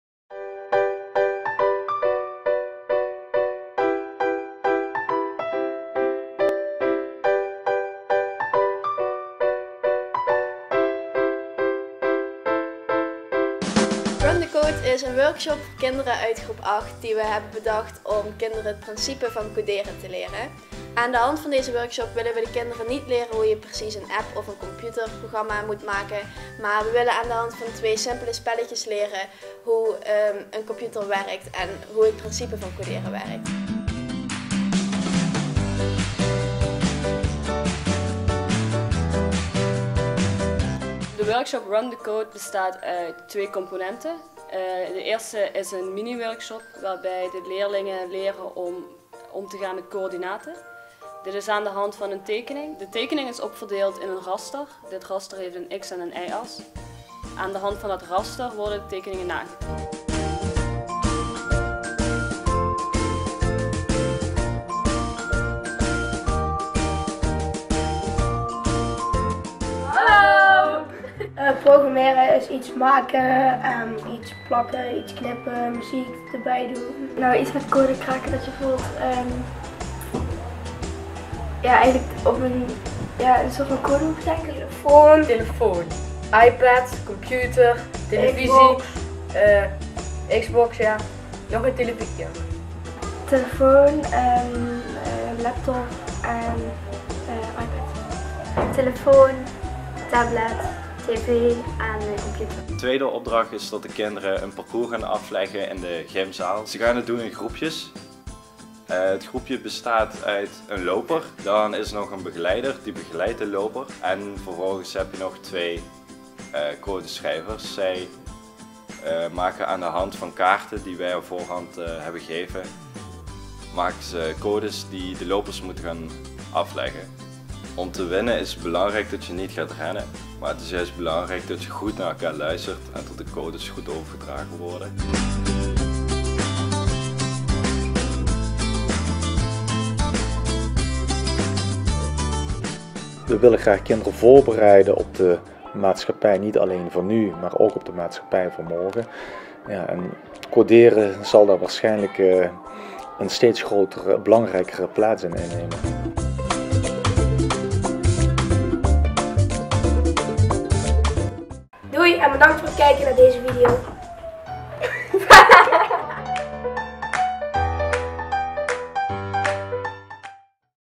FLEX || Reportage Run the Code
Run the Code is een workshop waarbij kinderen van groep 8 kennis maken met het principe van coderen.